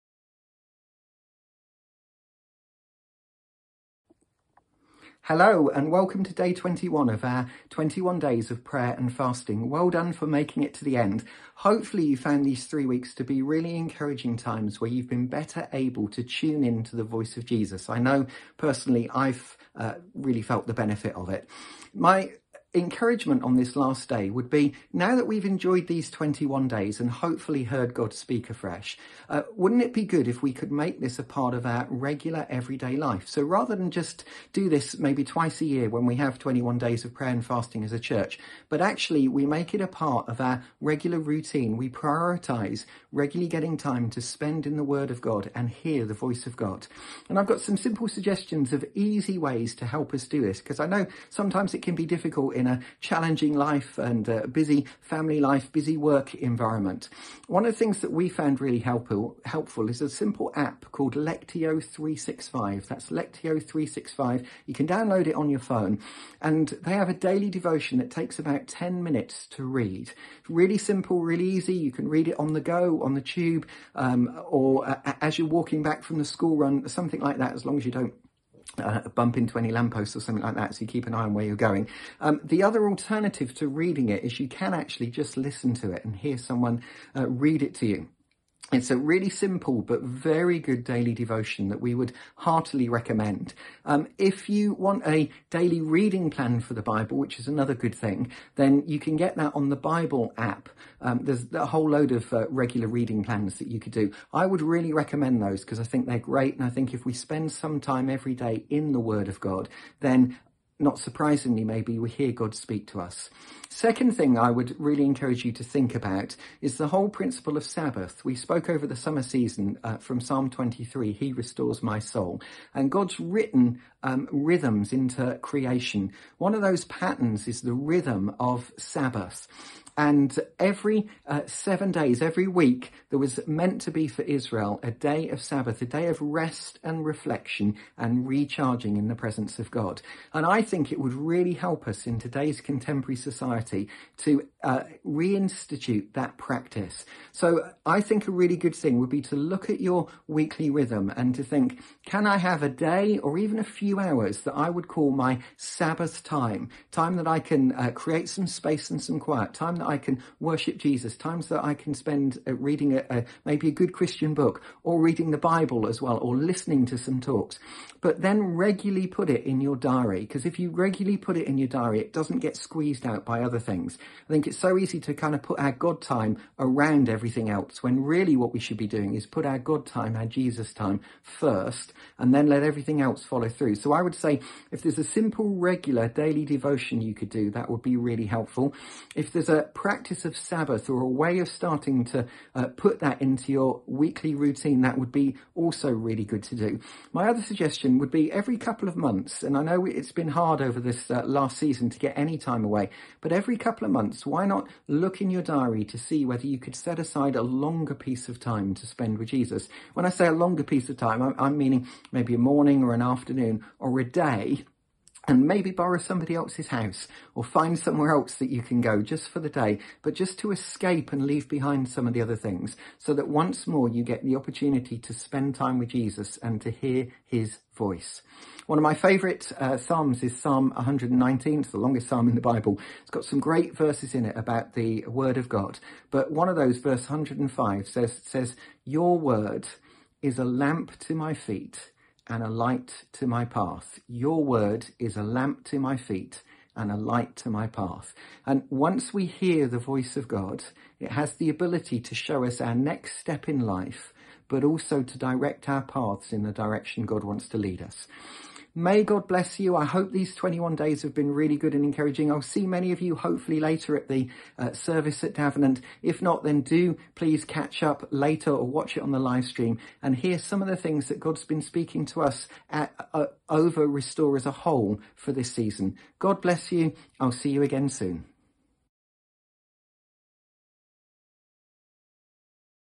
Day 21 Daily Devotion - 21 Days of Prayer and Fasting - Tuning in to Jesus - Sept 2021
Welcome to our final daily devotion, Day 2️⃣1️⃣ of our 21 Days of Prayer and Fasting.

We pray that you have been able to 'Tune in to Jesus' over these 21 Days, that it has been helpful to lay aside other voices and distractions so that you can hear God speaking more clearly, that you have been blessed and been a blessing.

Today as we break the fast, Ian encourages us not to leave prioritising time with God until our next 21 days but to make it a rhythm of our life, to read His Word and meditate on it, and to take regular Sabbath rest and time with Jesus.

Let's keep encouraging one another to Tune in to Jesus! Remember, God is always speaking, so let's make the space and time in our lives to listen to Him...

Week 3 Day 21 Ian King   SD 480p